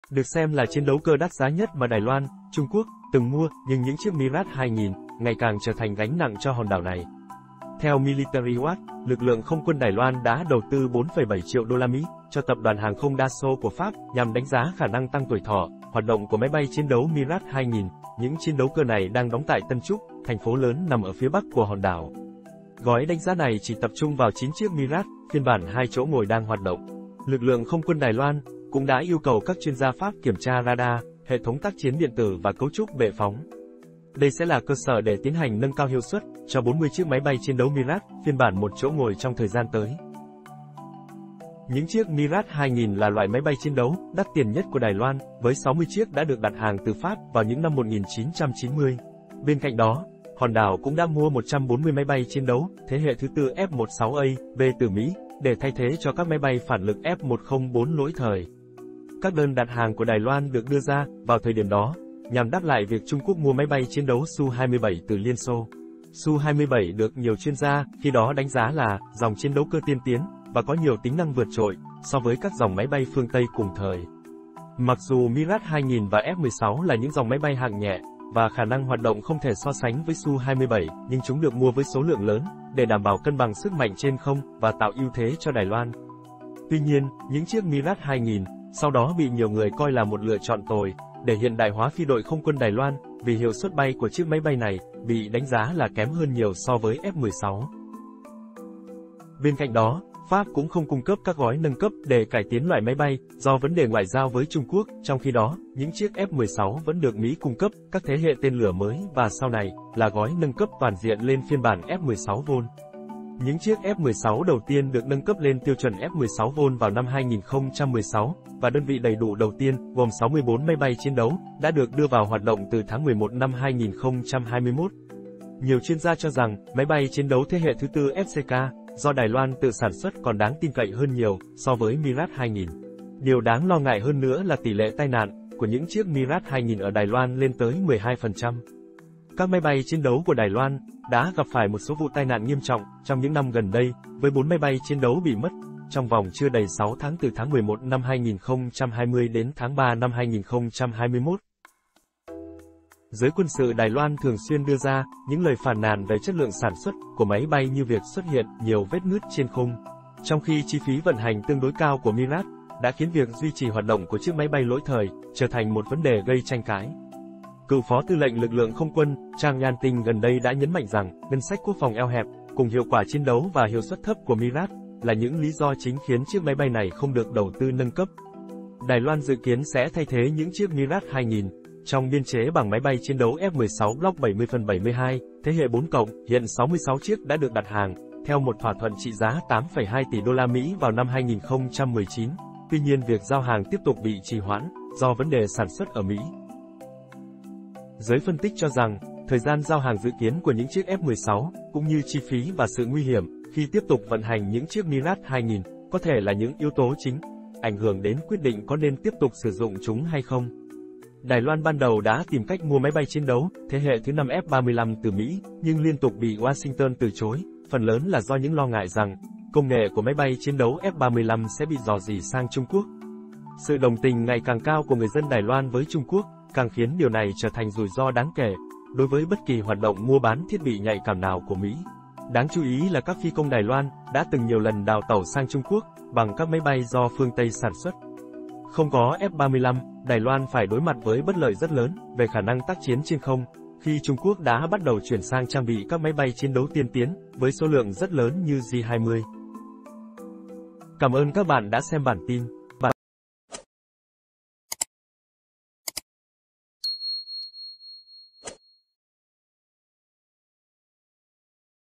ĐÀI LOAN VỚI GÁNH NẶNG MIRAGE 2000 CỦA PHÁP
Theo Military Watch, lực lượng không quân Đài Loan đã đầu tư 4.7 triệu USD, cho tập đoàn hàng không Dassault của Pháp, nhằm đánh giá khả năng tăng tuổi thọ, hoạt động của máy bay chiến đấu Mirage 2000, những chiến đấu cơ này đang đóng tại Tân Trúc, thành phố lớn nằm ở phía bắc của hòn đảo.